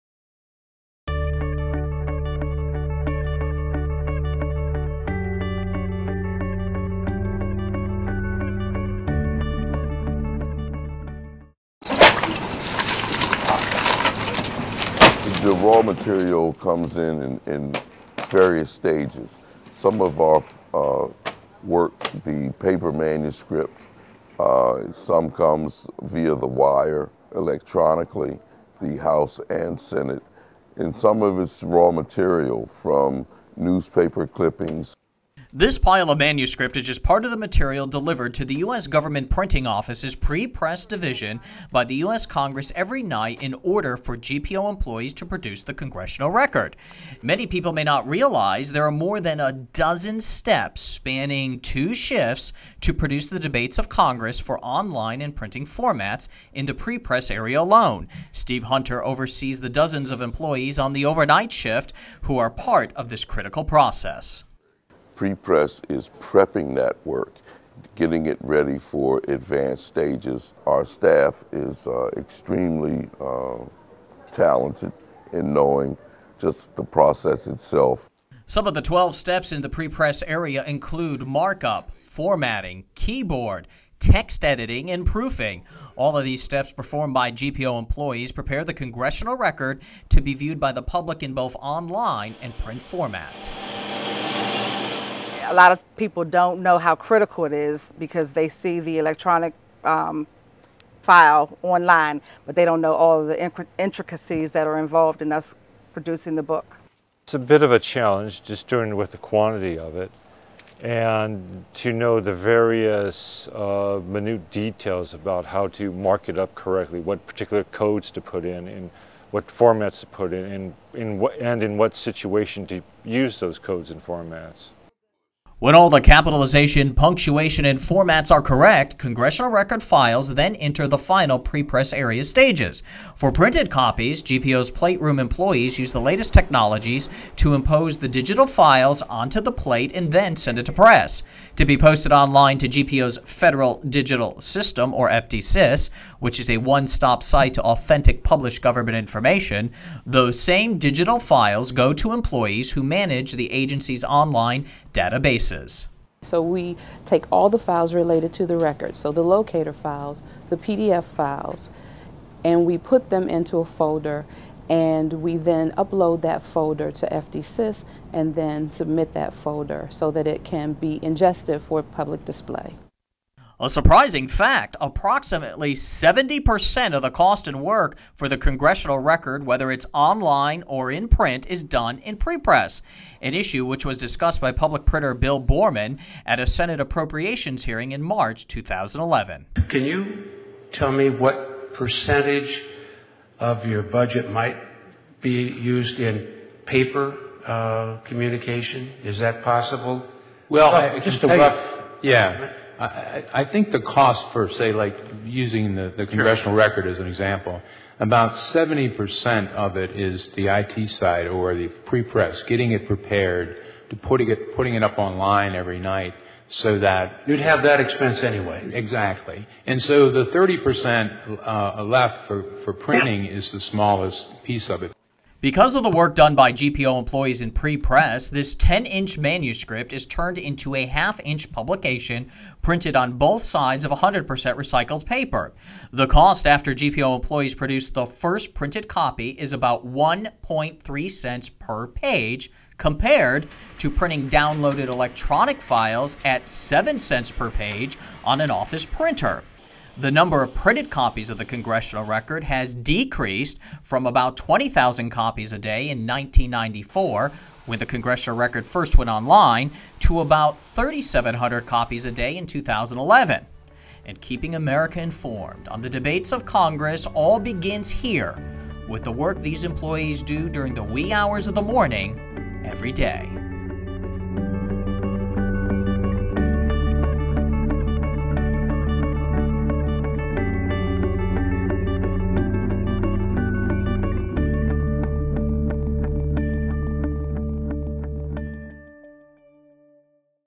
U.S. Government Printing Office: Production of the Congressional Record
April 2011---Employees at the U.S. Government Printing Office (GPO) are responsible for producing the debates of Congress in both online and print formats.  GPO employees working in the early morning hours take an 10 inch raw manuscript and turn it into a half inch printed copy and digital files to be viewed online on GPO's Federal Digital System Nearly 70 percent of the cost in producing the Congressional Record is in prepress--getting it ready to be posted online and printed.